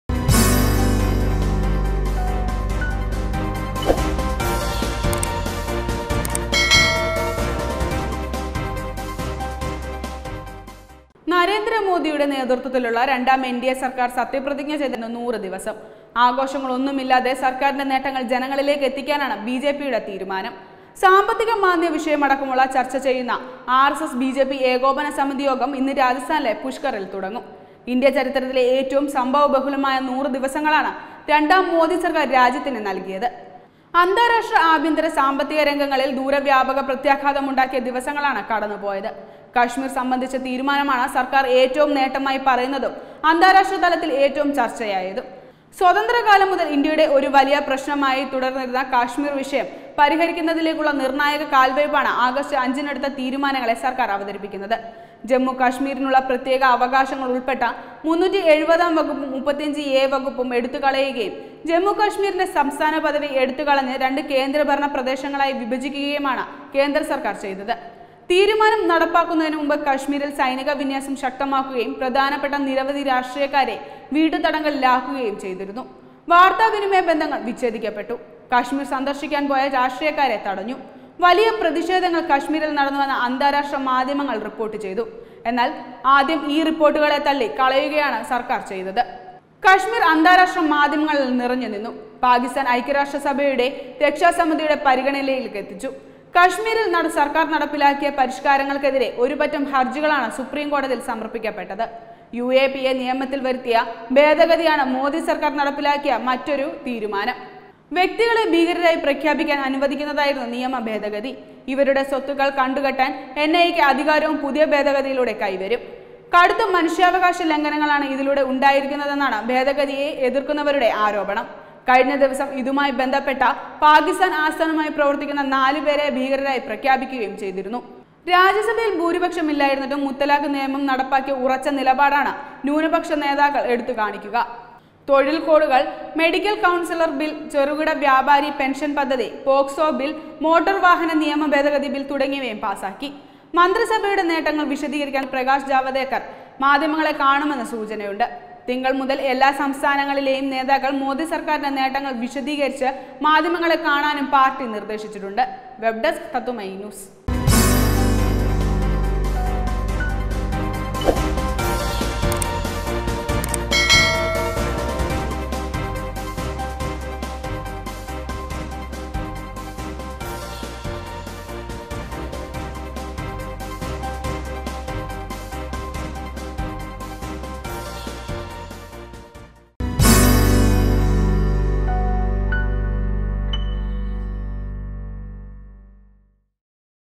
നൂറു ദിനം - നൂറു കണക്കിന് നേട്ടങ്ങൾ
നരേന്ദ്ര മോദിയുടെ നേതൃത്വത്തിലുള്ള രണ്ടാം എൻഡിഎ സർക്കാർ സത്യപ്രതിജ്ഞ ചെയ്തിട്ട് ഇന്നു 100 ദിവസം. ആഘോഷങ്ങളൊന്നുമില്ലാതെ സർക്കാരിന്റെ നേട്ടങ്ങൾ ജനങ്ങളിലേക്കെത്തിക്കാനാണു ബിജെപിയുടെ തീരുമാനം. സാമ്പത്തികമാന്ദ്യ വിഷയങ്ങളടക്കം ചർച്ച ചെയ്യുന്ന ആർഎസ്എസ് – ബിജെപി ഏകോപന സമിതി യോഗം ഇന്നു രാജസ്ഥാനിലെ പുഷ്കറിൽ തുടങ്ങും.